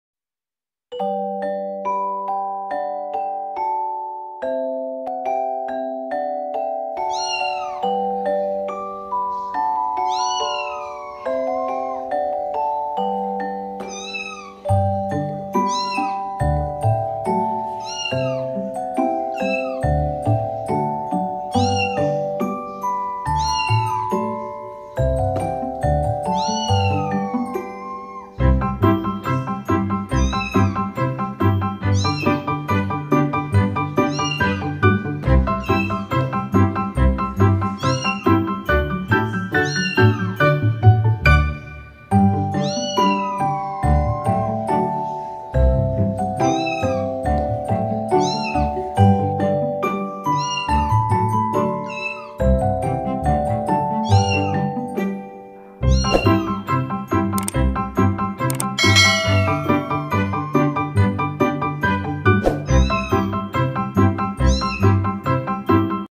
CAT TV: Catch me if you can
A very smart cat showing the proper way of catching a ball